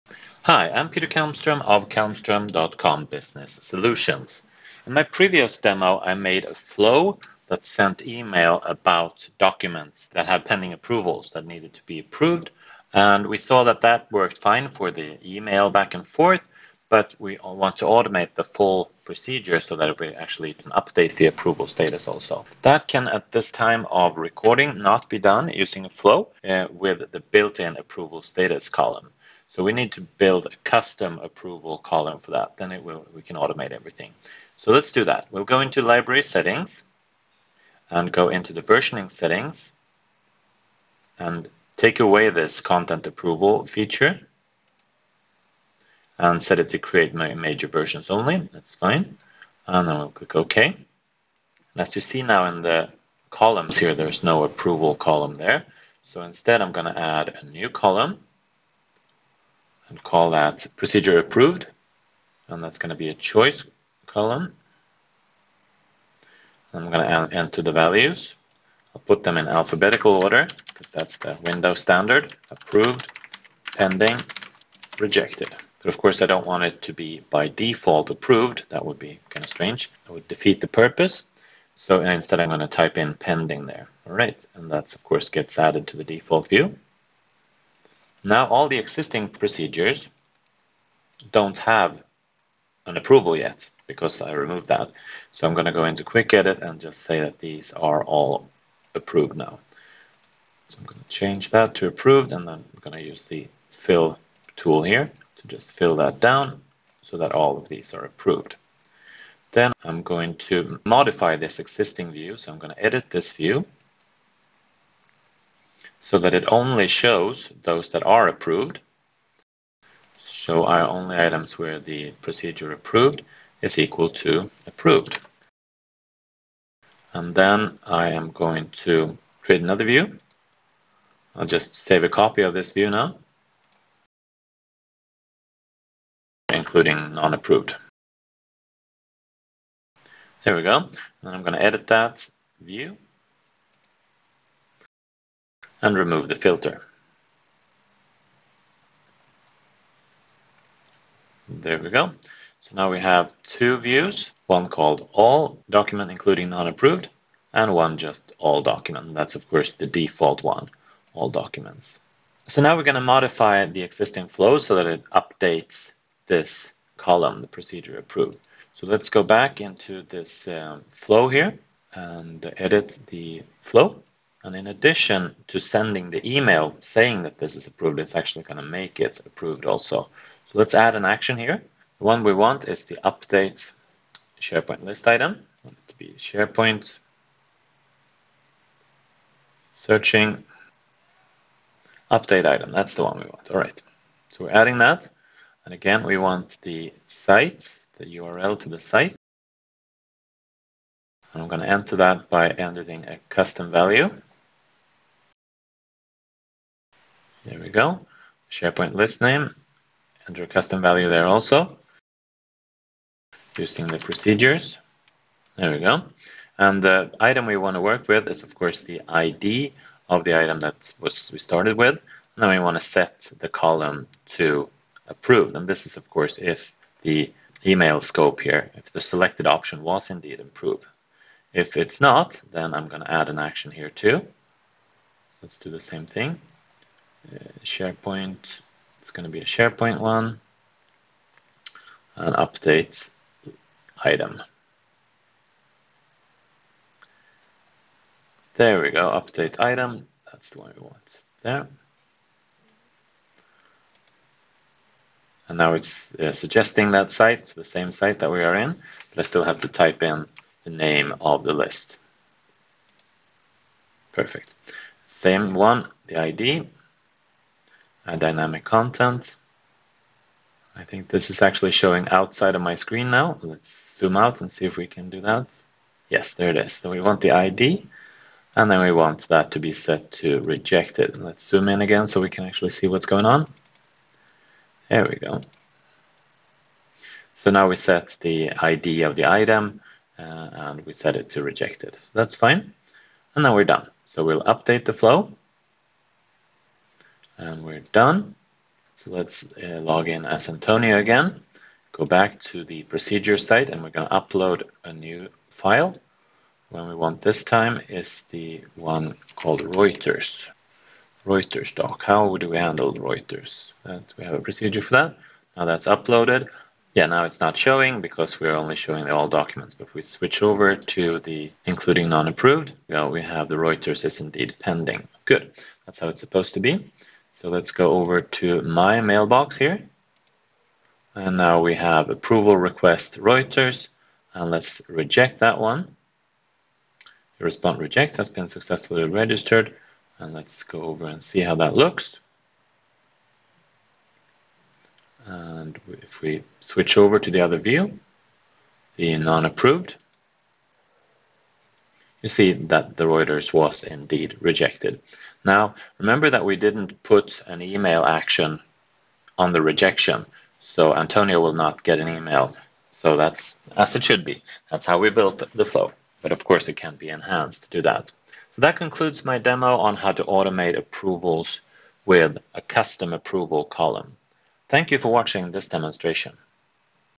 Approvals SharePoint Flow with a custom status column
Create a flow that sends an approval e-mails and updates a custom approval status column